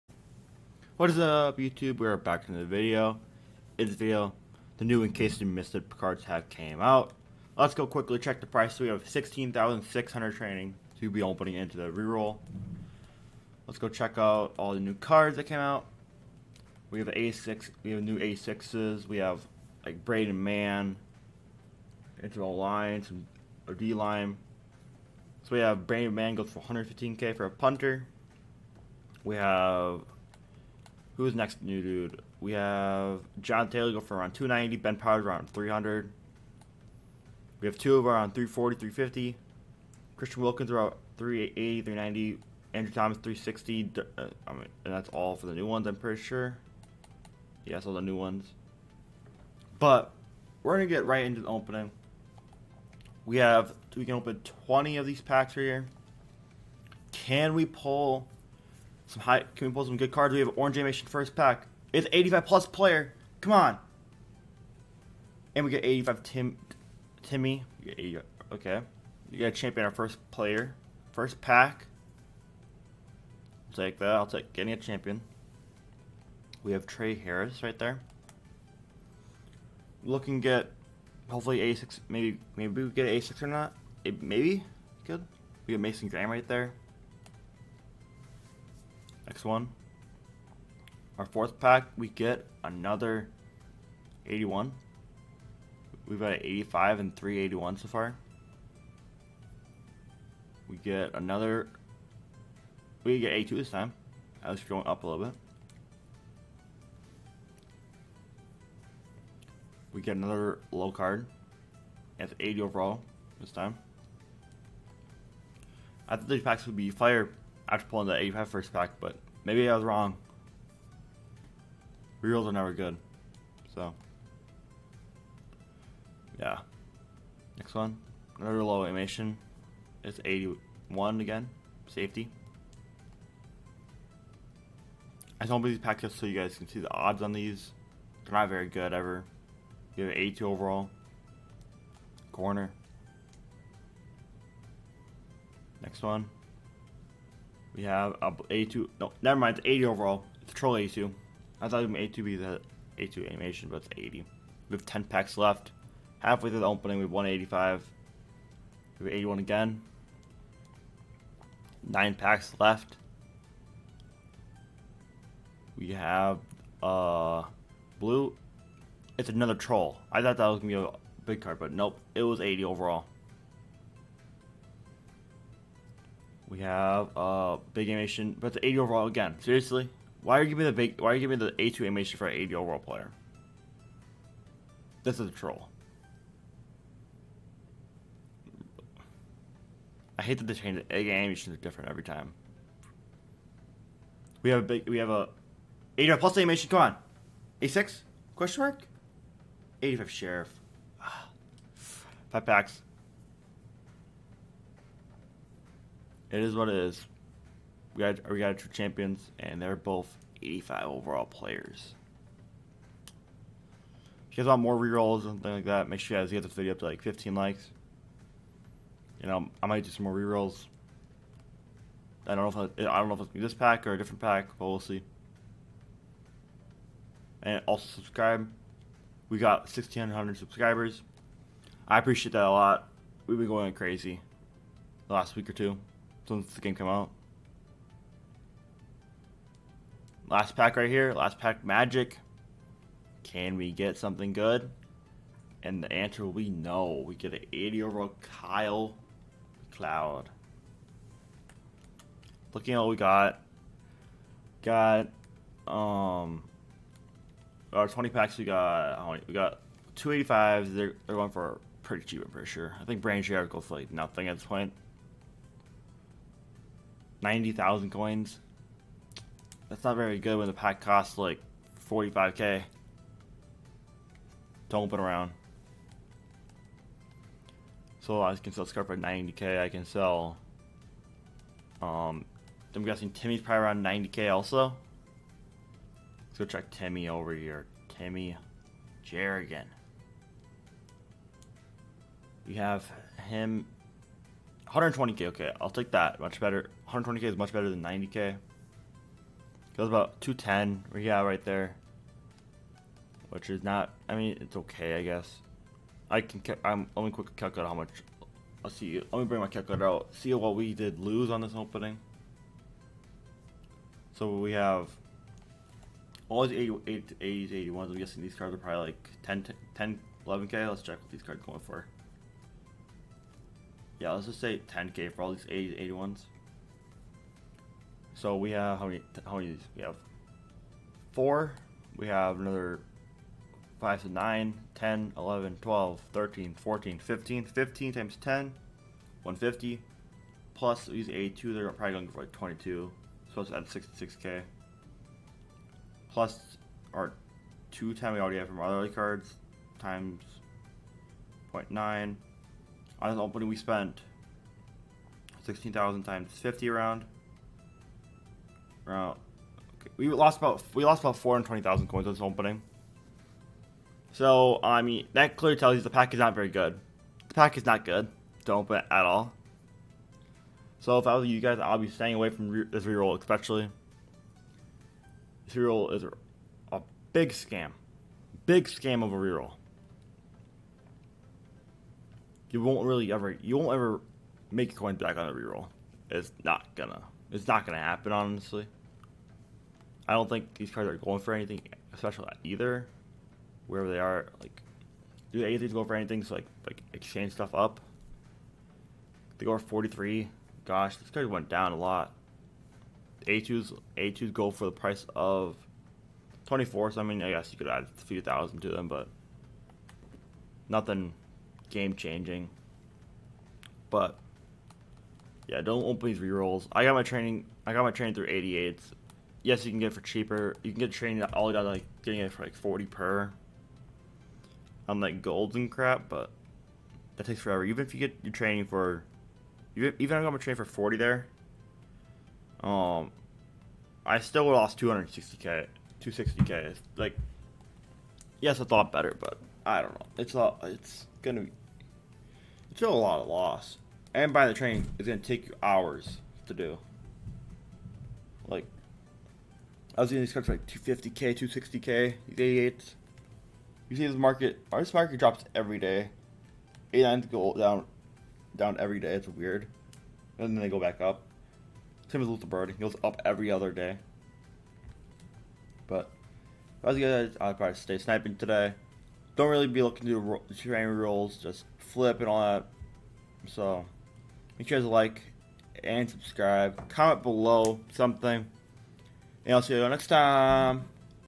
ICYMI TRAINING PACK OPENING! CAN WE PULL A 86 OVR!?
I test out the ICYMI training pack trying to see if its worth opening or not!